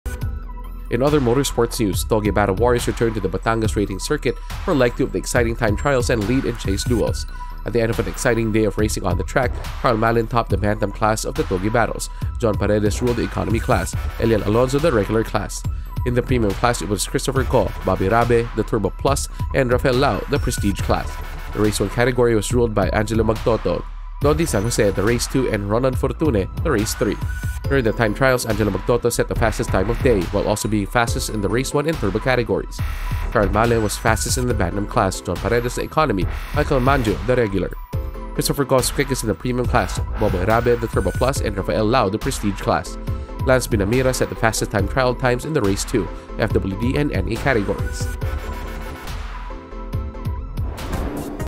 FTW’s Touge Battle Leg 2 | Motorsports News
Touge Battle warriors returned to the Batangas Racing Circuit for Leg 2 of exciting time trials and lead-and -chase duels.

At the end of an exciting day of racing on the track, Karl Mallen topped the Bantam Class of the Touge Battles; John Paredes ruled the Economy Classs; Eliel Alonzo the Regular Class.